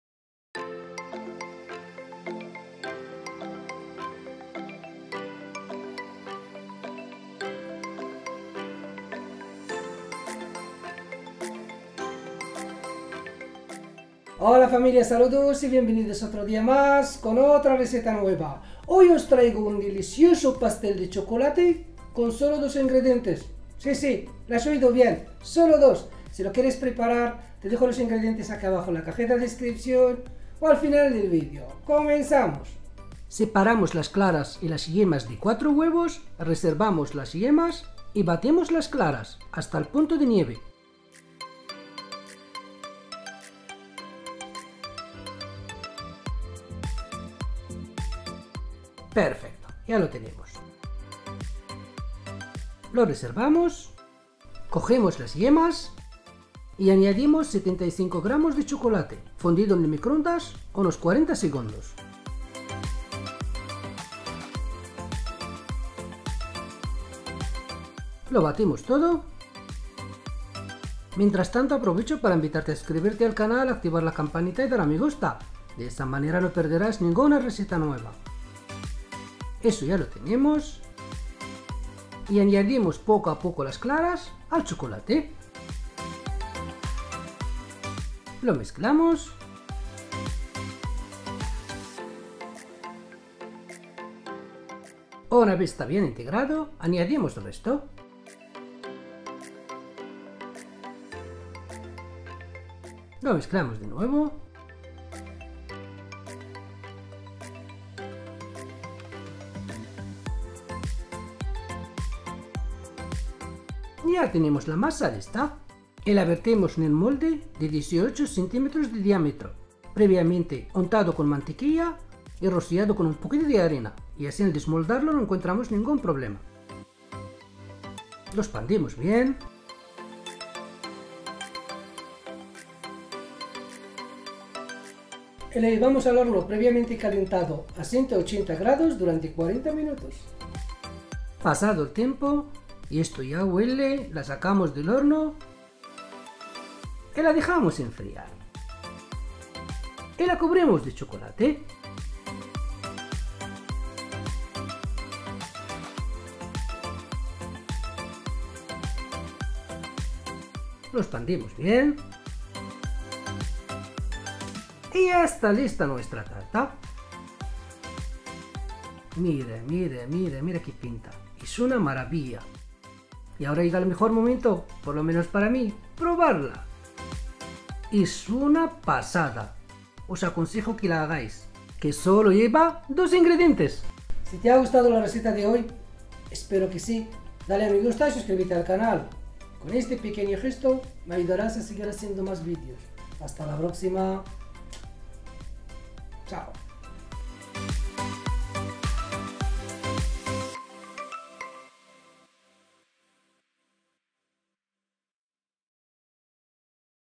PASTEL DE CHOCOLATE con solo 2 INGREDIENTES | ¡Es una auténtica pasada! | TAZI OFICIAL
Hoy os traigo un delicioso pastel de chocolate que es de los mejores que he probado ¿Y lo mejor? Solo necesitaréis dos ingredientes, sí, sí ¡Solo dos! Os animo a prepararlo y si tenéis cualquier duda os leo en los comentarios.

INGREDIENTES

4 Huevos
375 gr. de Chocolate negro